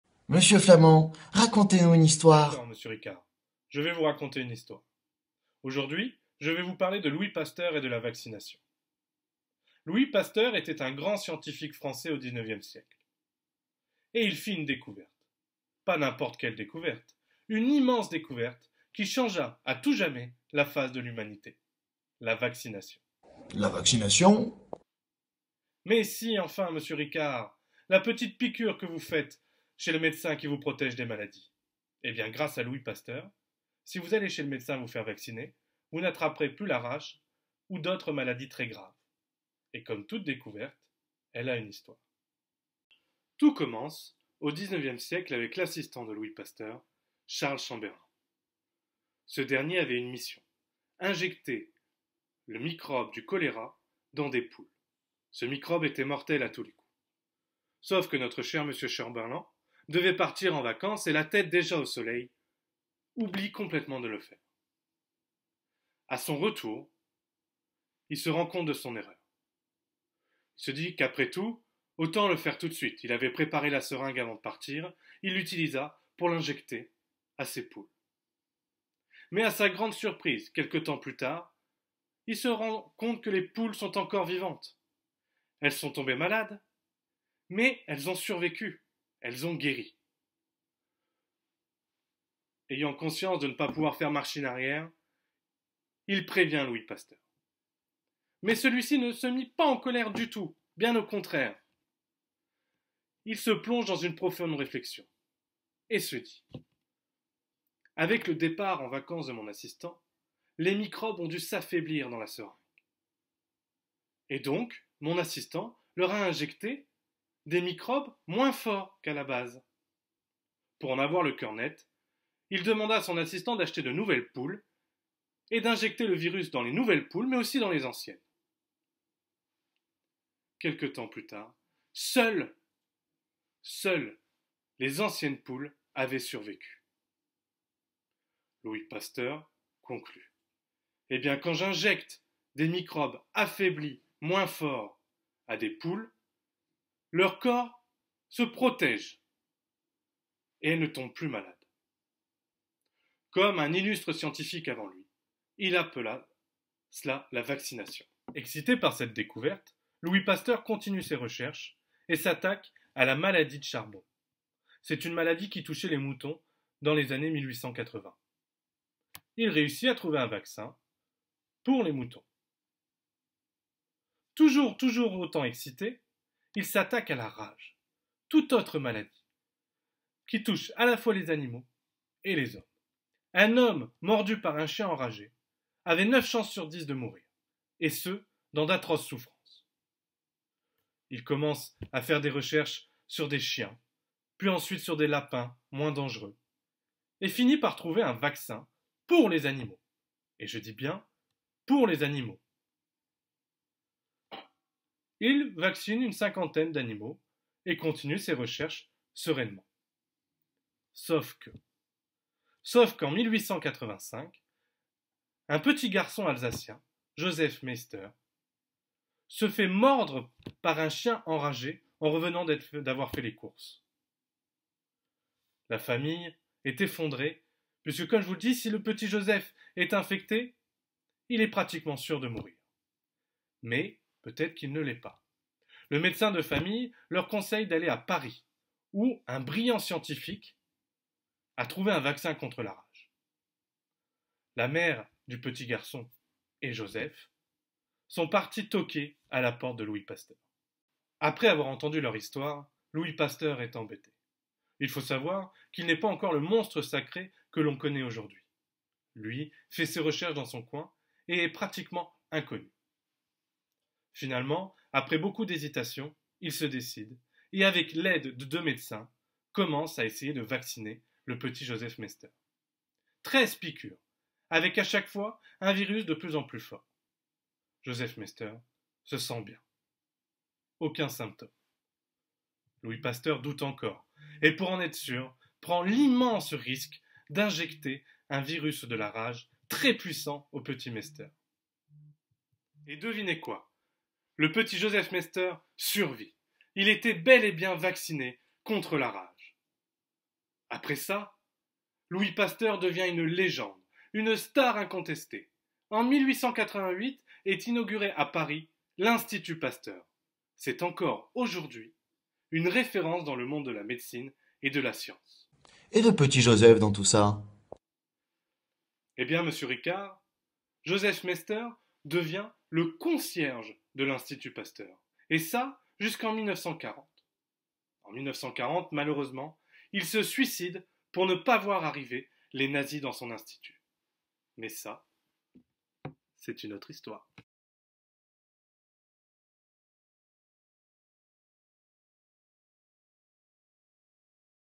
Louis Pasteur et la vaccination
*** L'instant culture ***
Dans ses petites histoires, Monsieur Flamant vous emmène à la rencontre de personnages historiques Français qui ont marqué l'histoire...
Notre premier grand-homme illustre : Louis Pasteur, et la vaccination.